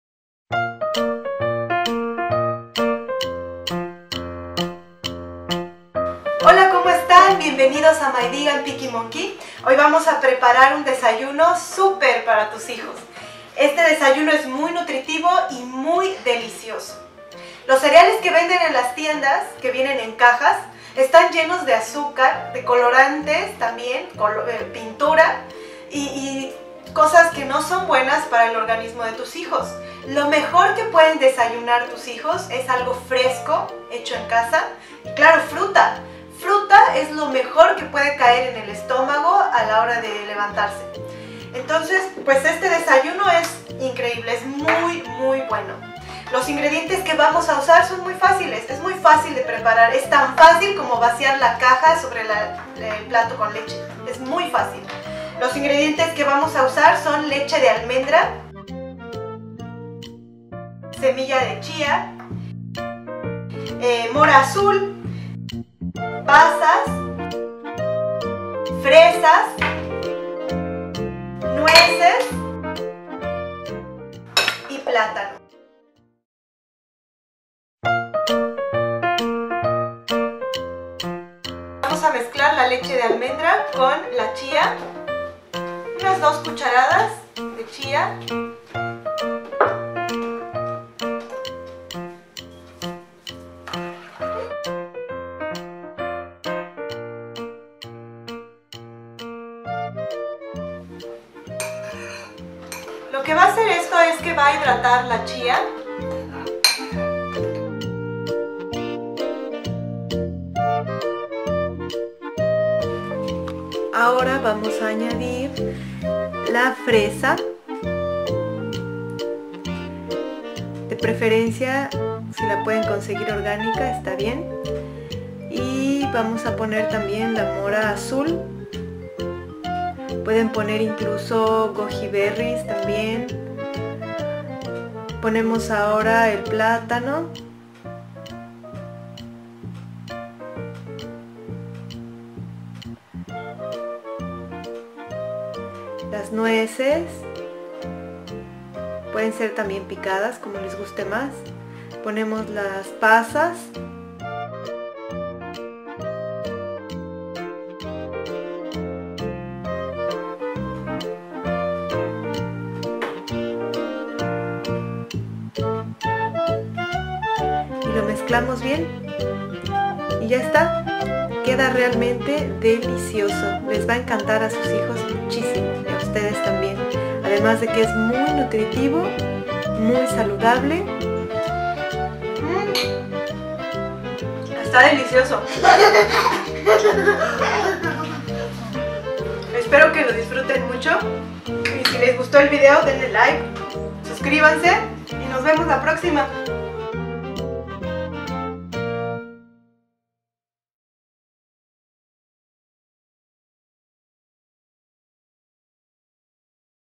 El mejor Desayuno para Niños, Pudin de chia/raw vegan
Cuidado! Muchos cereales de caja aportan 7 cucharadas y media de  azúcar en el desayuno de un niño ya que varias marcas son mitad azúcar y mitad "colorantes, saborizantes, endulzantes, conservadores etc etc. Sin contar que especialmente los cereales para niños  son los que menos fibra ofrecen.
A esto habra que sumar el precio tan elevado ya que es mas barato consumir avena con plátano o fruta picada.
Por eso aquí les dejamos una idea de lo que pueden hacer si se proponen darles a sus hijos de lo mejor.